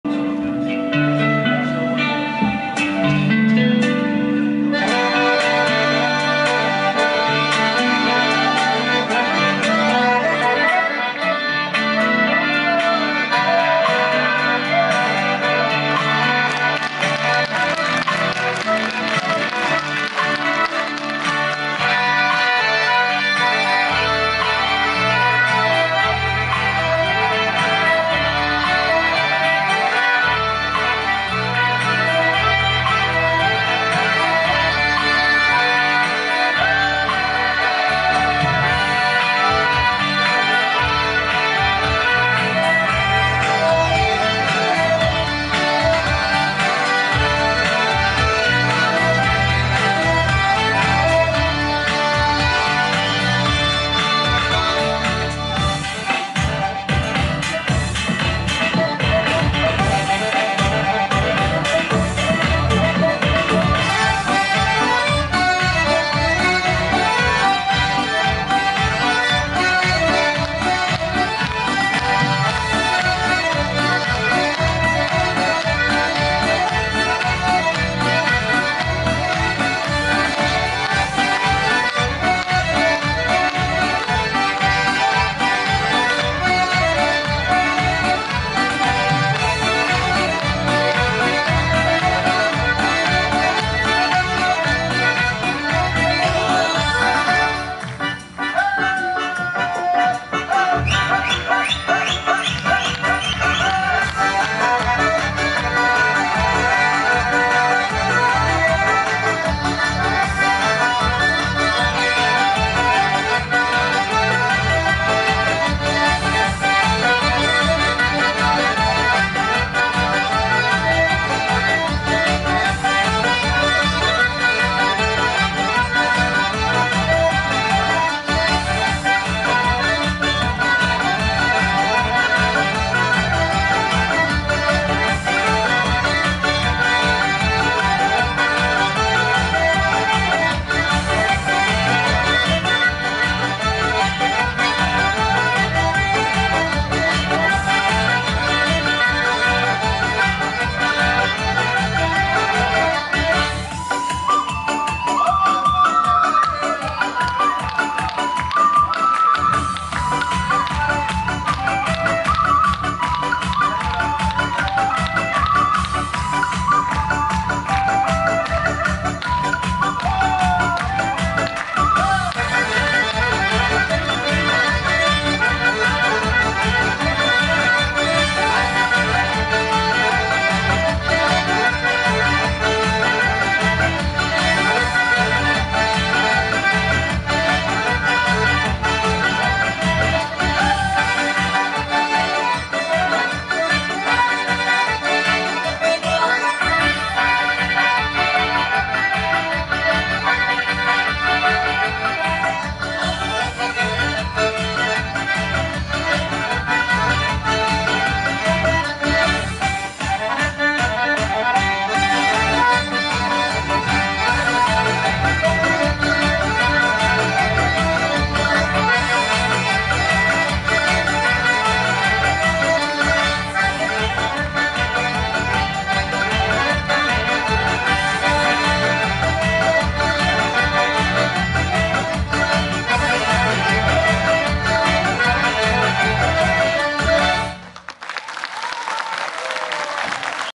Зал в городе Бавлы сильно оживился когда вышли гармонисты из БАВЛОВ !!!
Бавлы 2017 год Фестиваль гармонистов !